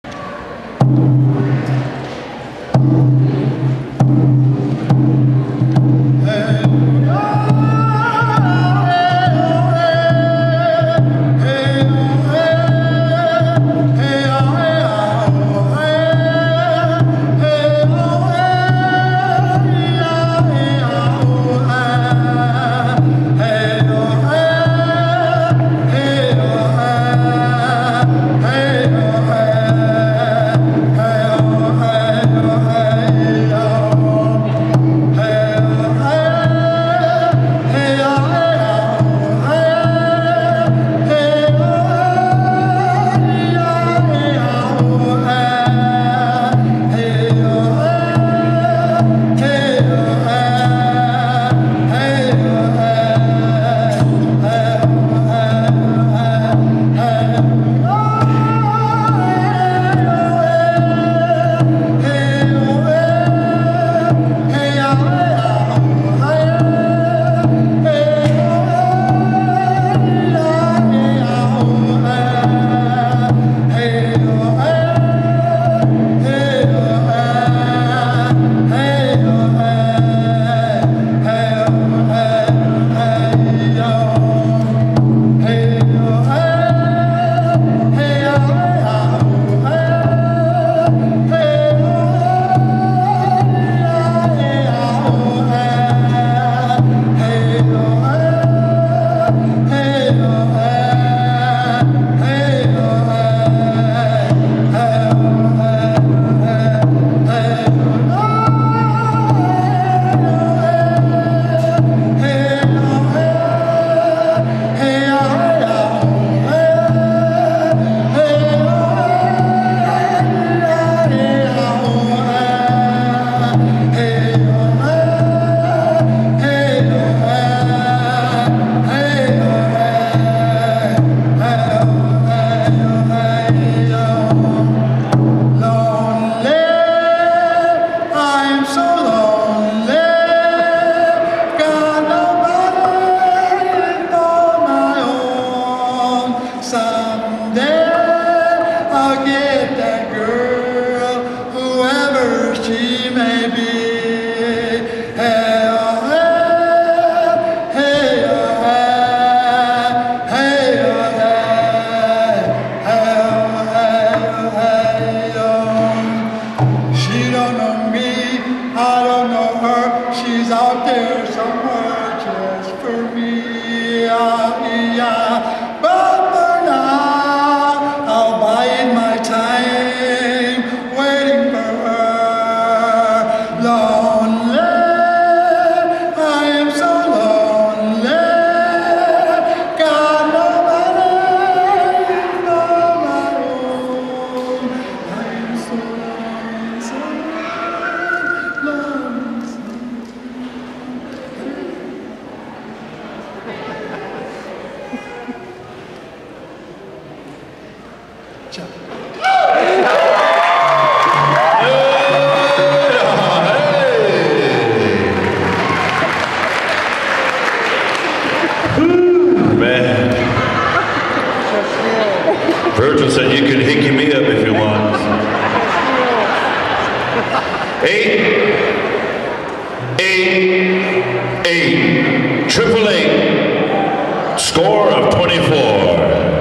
Sonny "Superman" Eaglespeaker 2nd place - 4th Annual Tanner Alber's Round Dance Memorial 2015
Singing his final contest song. Sonny "Superman" Eaglespeaker 2nd place winner at the 4th Annual Tanner Alber's Round Dance Memorial 2015